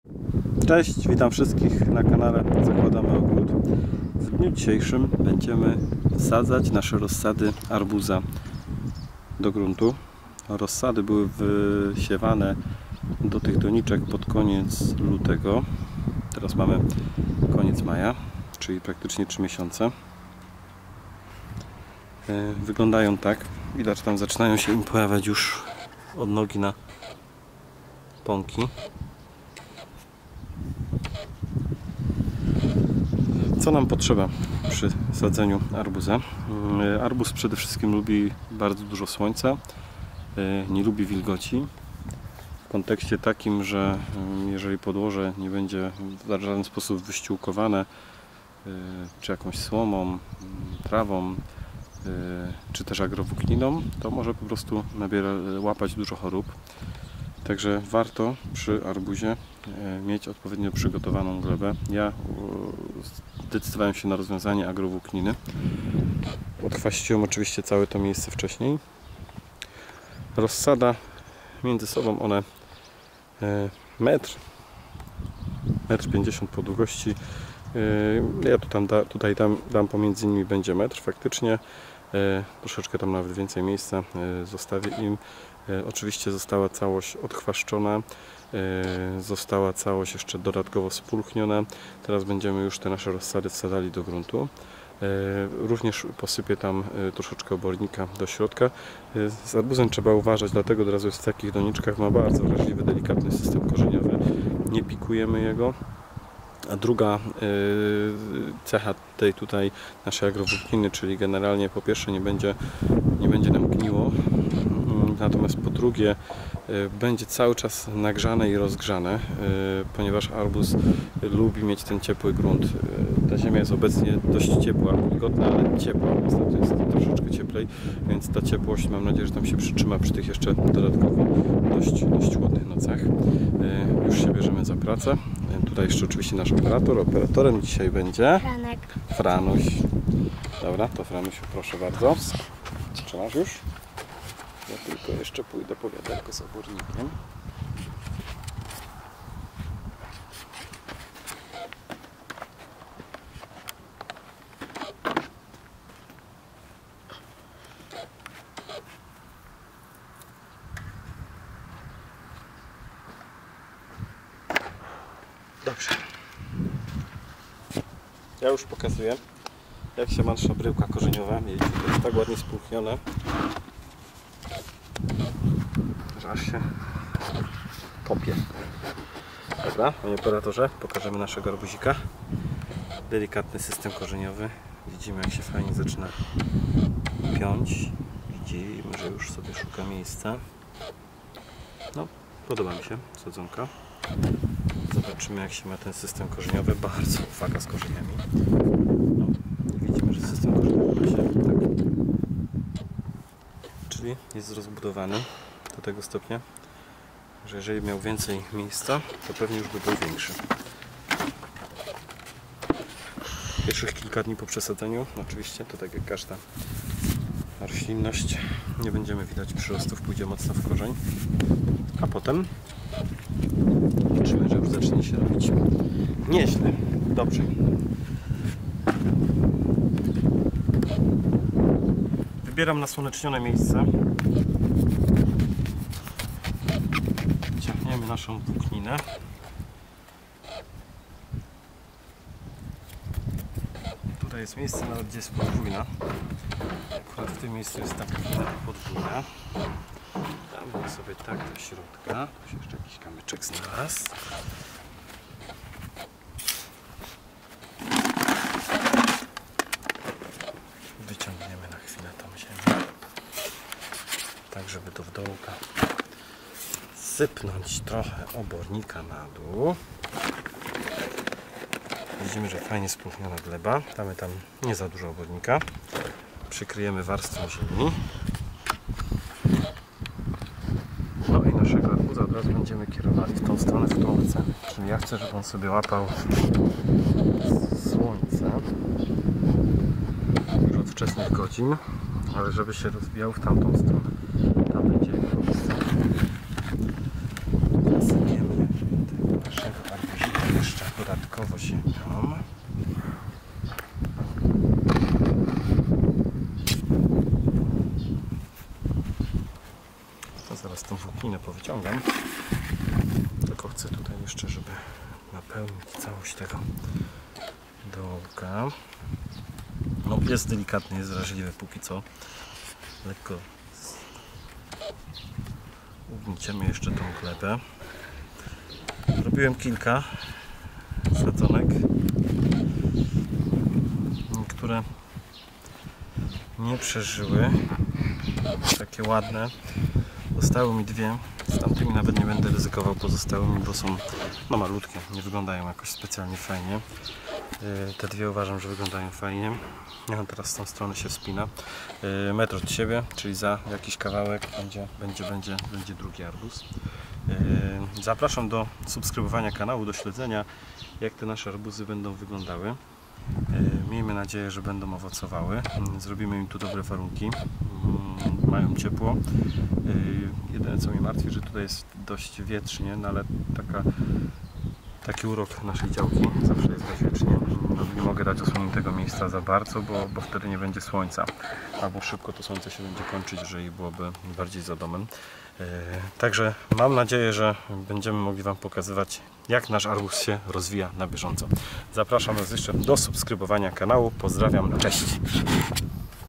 Jak posadzić arbuza? Arbuz sadzenie rozsady arbuza do gruntu.
Sadzenie rozsady arbuza do gruntu.

Arbuz bardzo lubi ciepło. Dlatego warto wyściółkować włuknina podłoże